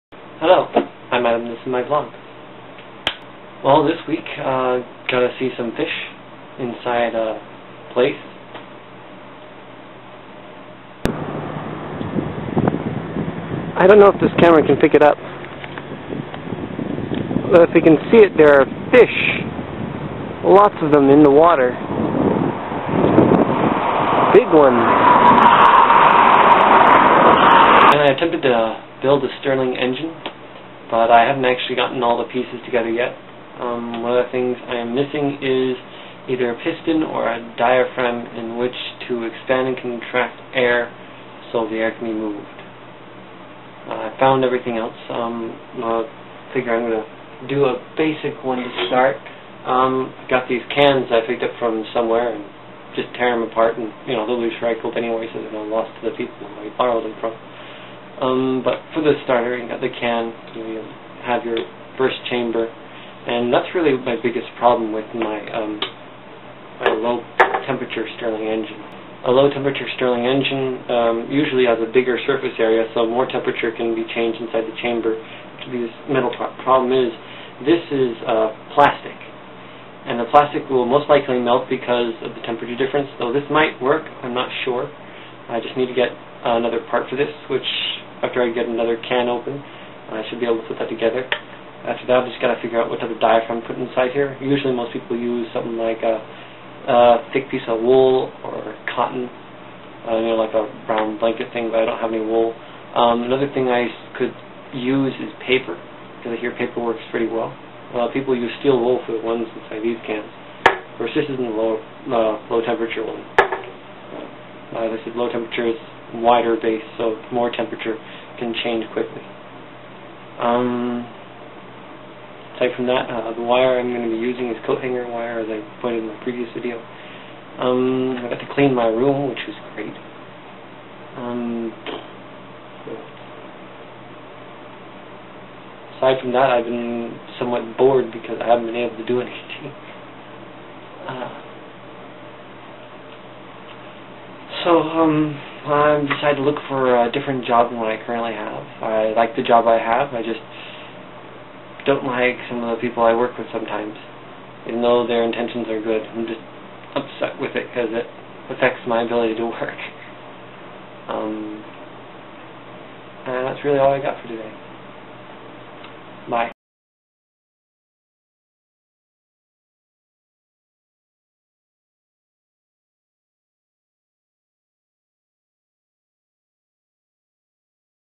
Fishy Plans
There are fish you can see. I talk more of the sterling engine show parts of the ones I'm working on. And I contemplate getting a new Job.